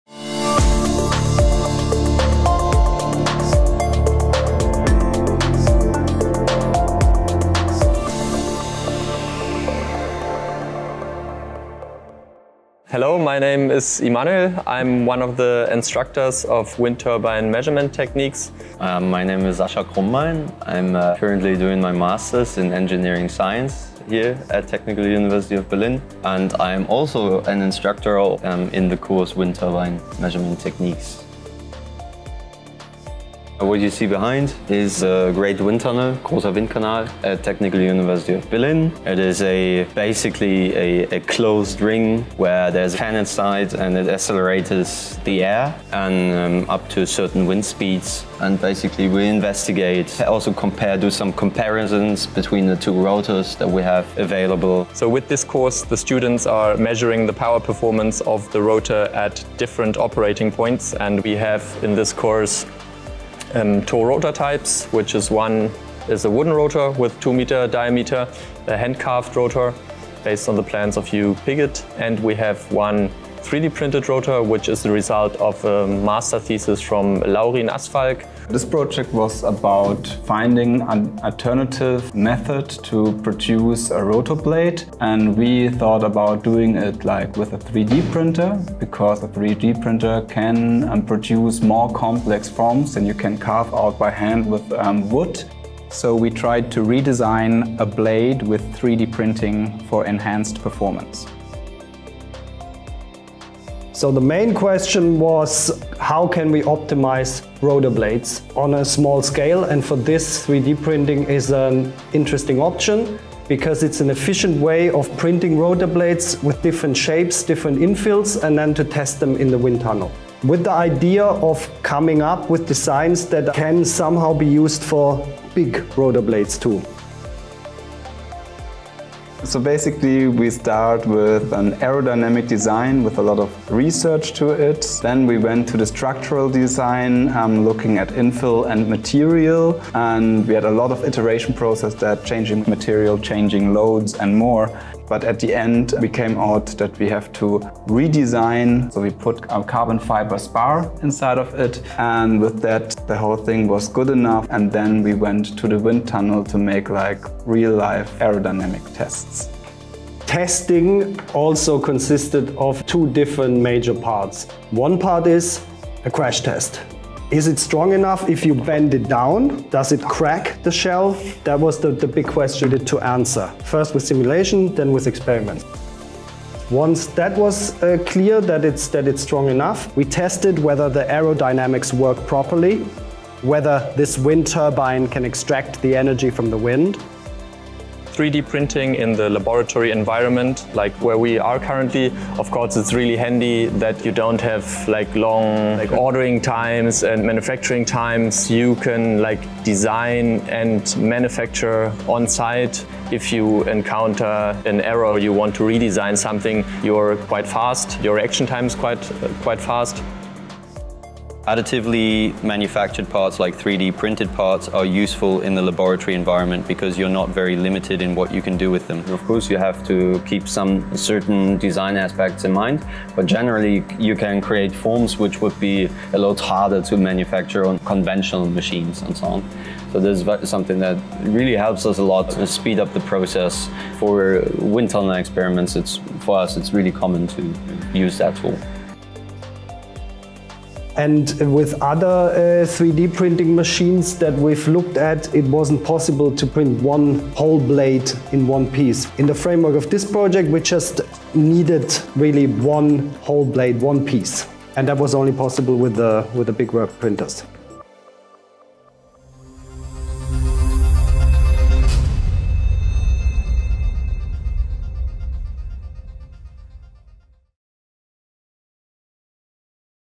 3D Printing Rotor Blades powers Wind Turbine Research | TU Berlin
On average, wind turbine blades are a massive 35 meters long. When it comes to reengineering these towering blades, no other technology offers the freedom, precision, and adaptability to scale parts quite like 3D printing. While replicating them in a university lab might be near impossible, a scaled prototype with 1 meter blades is very much in the wheelhouse of a large-build volume 3D printer. Here, researchers go big by starting small.

Based on 3D-printed rotor blades, Technische Universität Berlin (TU Berlin) offers a course - Wind Turbine Measurement Techniques that imparts skills to measure the performance of the blades at different operating points. The students learn how to gauge the speed of the wind while at the same time assess the power generated by the turbine. The course revolves around comparing the performance of a traditionally made, hand-carved, 2 meter wooden blade with a 3D-printed 1 meter rotor blade with gyroid infill.

The additively manufactured blade is the fruit of the research conducted by a Ph.D. and Master’s student who studied at TU Berlin, Jörg Abler, and Laurin Assfalg. During the study, they discovered that with 3D printing, experimenting with different infills, shapes, and materials, the sky's the limit.